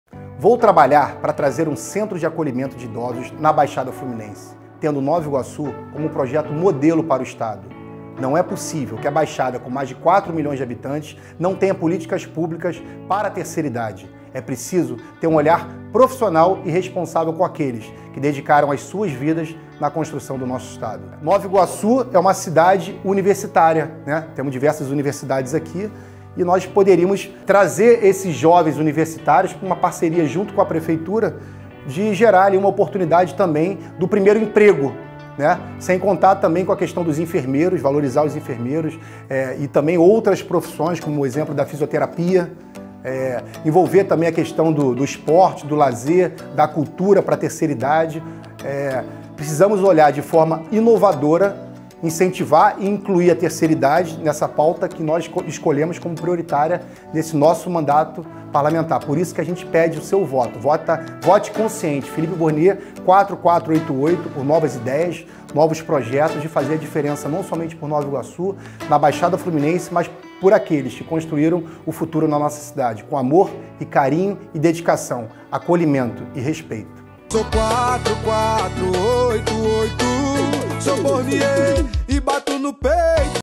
Eu quero trazer um Centro de Acolhimento de Idosos para a Baixada Fluminense!
Precisamos olhar de forma diferente para conseguir realizar projetos que funcionem de verdade.
Eu quero trazer um Centro de Acolhimento de Idosos aonde o atendimento seja realizado em parceira com Universidades e Prefeituras, integrando quem precisa de apoio com quem quer oportunidades de trabalho.

Veja mais no vídeo.
Peço o seu voto para tornar essa ideia uma realidade.